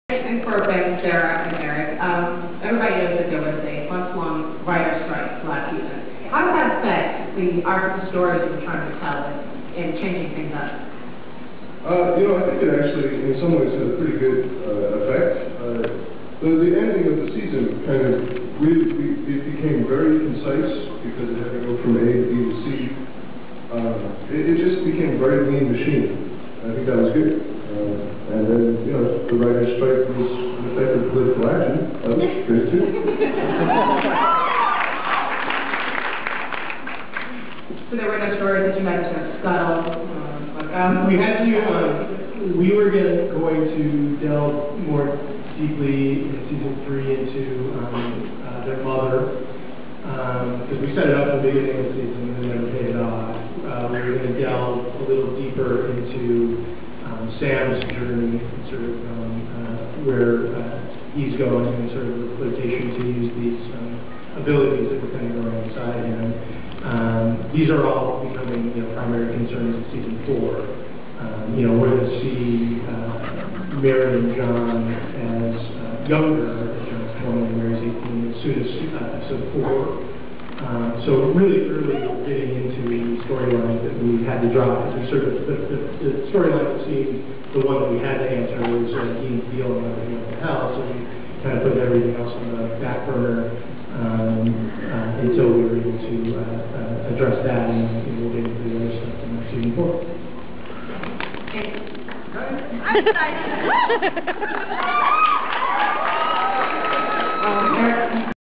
SPN at SDCC 08: How the strike affected Season 3 (Spoiler)
A clip from the Supernatural panel held on 7/27/08 at the San Diego Comic Con. This clip is slightly spoilery because they discuss something that they were thinking about for season 3 that will instead be in season 4.